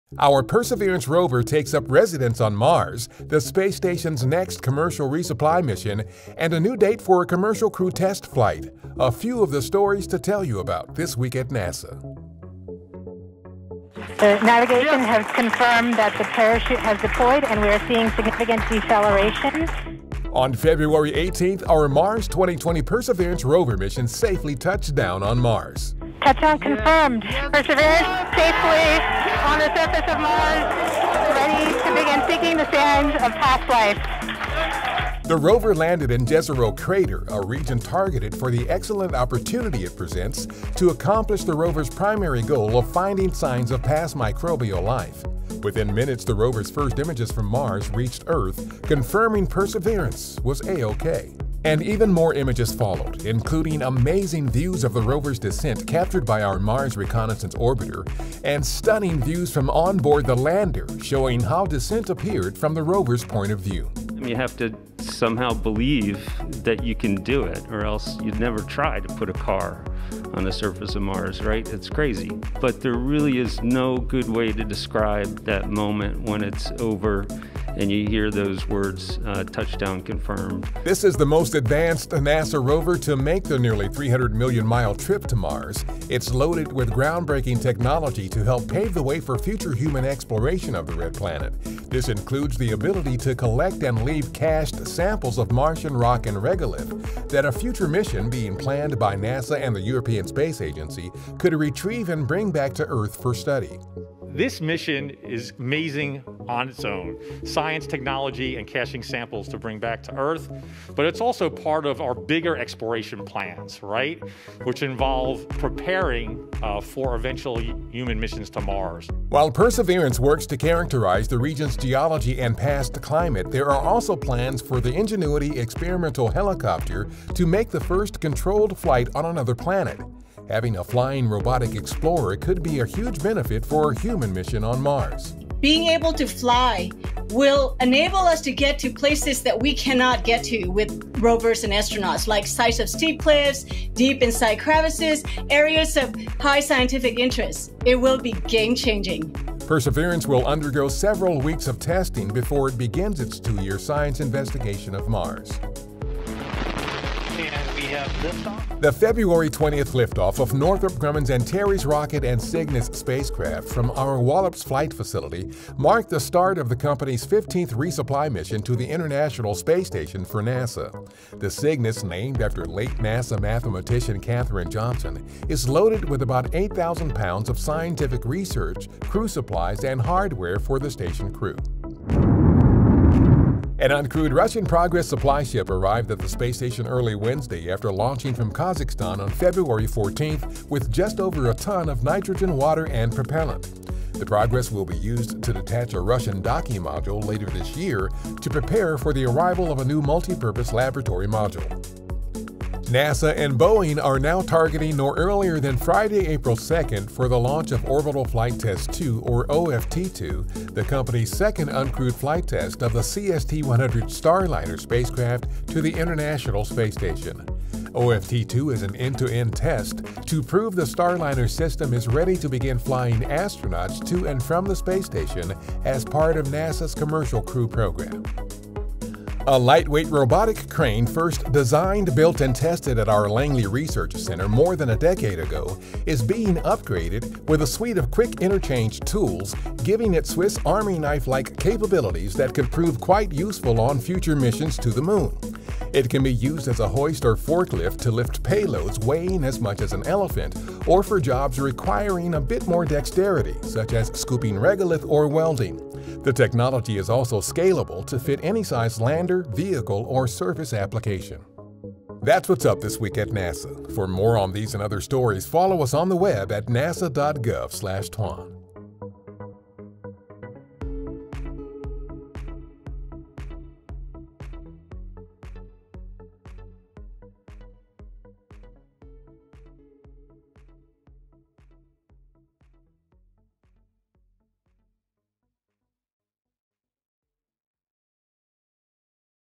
Our Perseverance Rover Takes up Residence on Mars on This Week @NASA – February 20, 2021
Our Perseverance rover takes up residence on Mars, the space station’s next commercial resupply mission, and a new date for a commercial crew test flight … a few of the stories to tell you about – This Week at NASA!